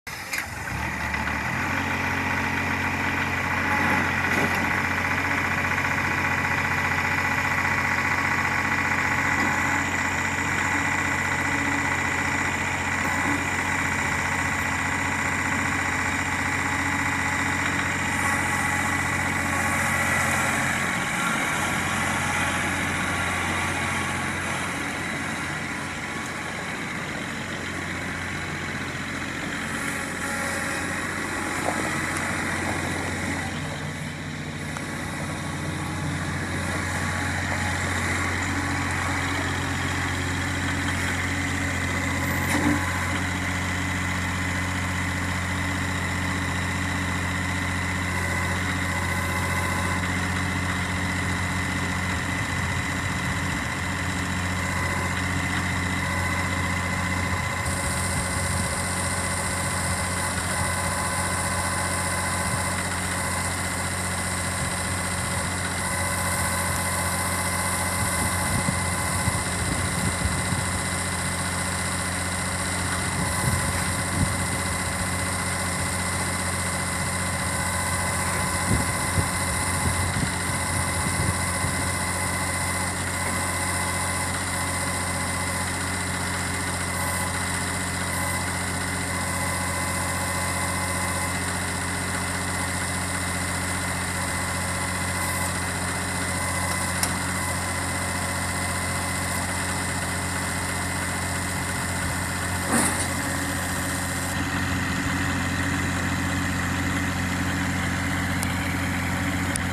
2017 KUBOTA L3301HST For Sale
OROPS, INDUSTRIAL TIRES, HYDROSTATIC TRANSMISSION, 4WD, DIESEL ENGINE, FRONT LOADER W/ COUPLER, BACKHOE, (SHOWING 303 HRS.), SN 59627 ST# 5887